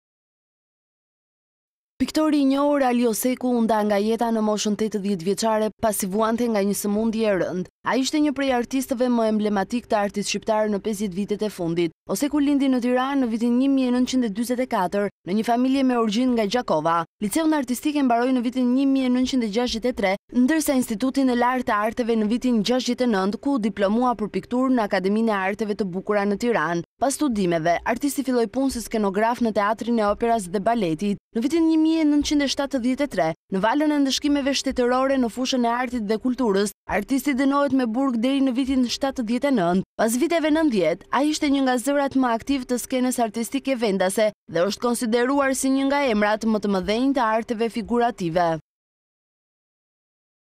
Ndahet nga jeta piktori Ali Oseku - Artisti vuante nga një sëmundje e rëndë | RTSH
Piktori i njohur Ali Oseku është ndarë nga jeta ditën e sotme. Oseku njihet si një prej artistëve më emblematik të artit shqiptar në 50 vitet e fundit.